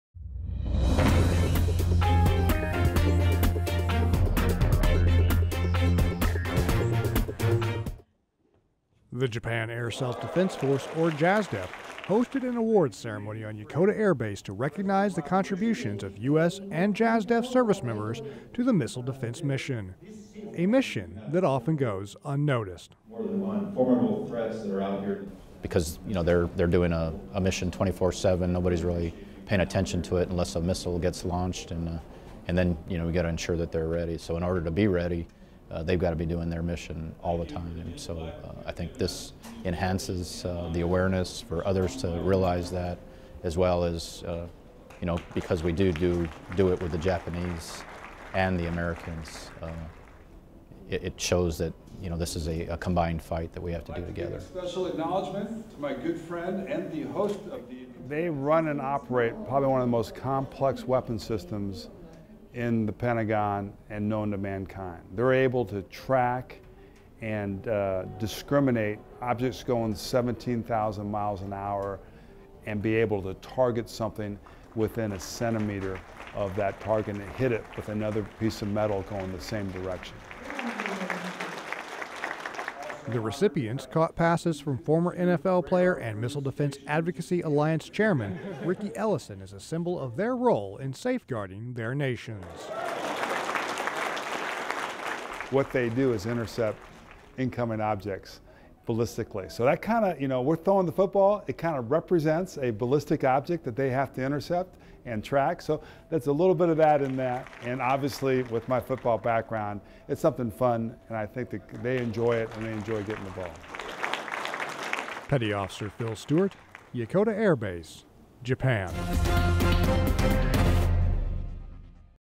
Yokota Air Base Hosts Missile Defender of the Year Award Ceremony
DescriptiThe military mission can be a difficult one, and service members are often recognized for the hard work they do, sometimes with medals, and other times with footballs.  Petty Officer Phill Stuart reports from Yokota Air Base where American and Japanese service members’ receiving skills were put to the test in a unique award ceremony…
on